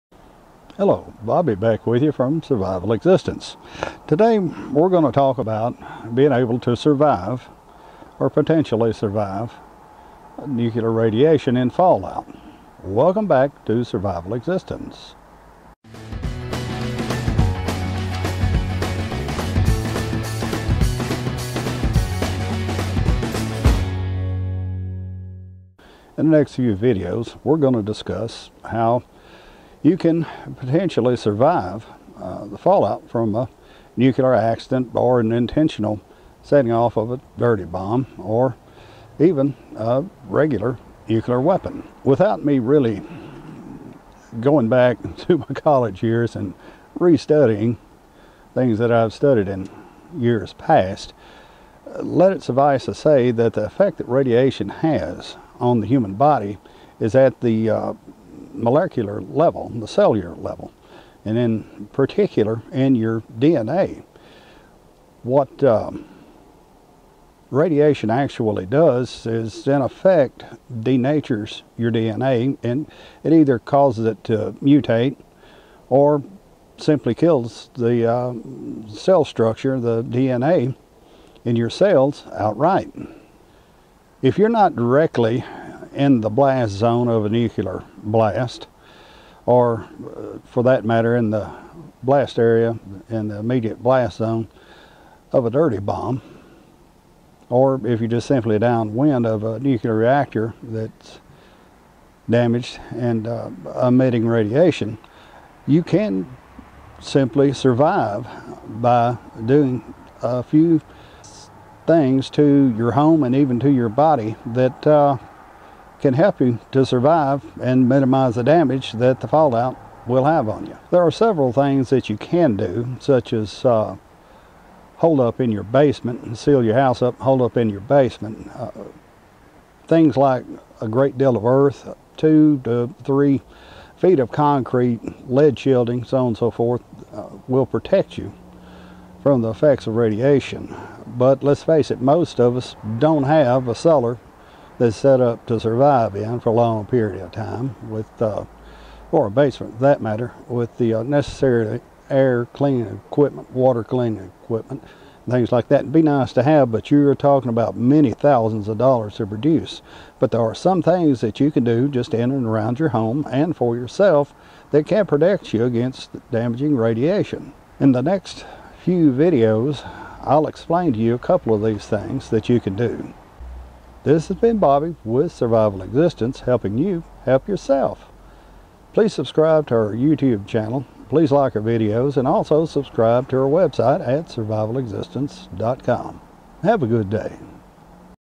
Surviving Radioactive Fallout
Preparing for nuclear fallout during a nuclear disaster can be done by taking a few precautions.